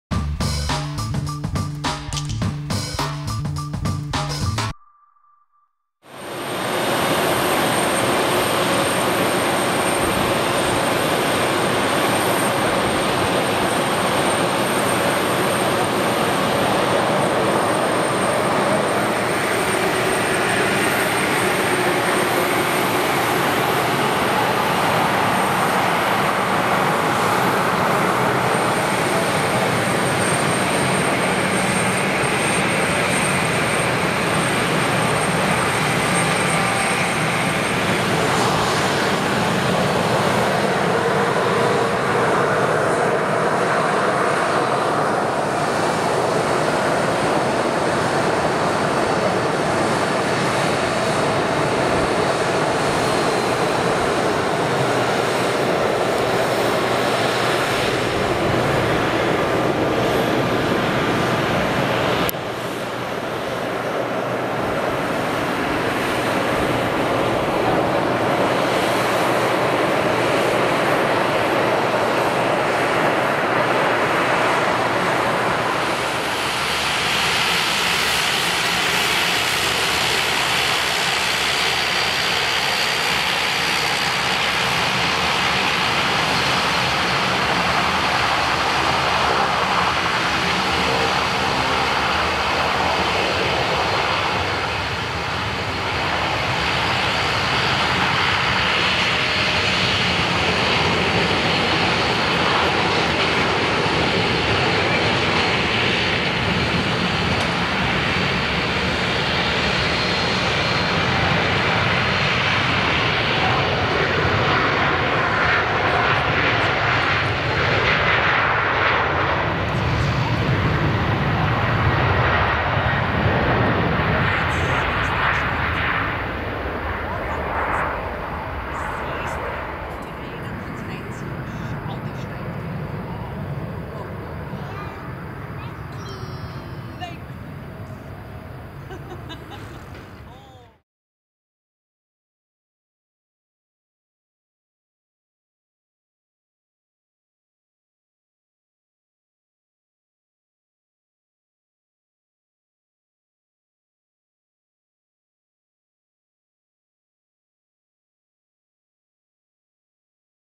Austrian Airlines Embraer 195 takeoff at Graz Airport | OE-LWO
Graz Airport - Frankfurt Airport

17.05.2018
Departure: 10.25
Arrival: 11.45

Embraer 195LR
OE-LWO
MSN: 555
First flight: unknown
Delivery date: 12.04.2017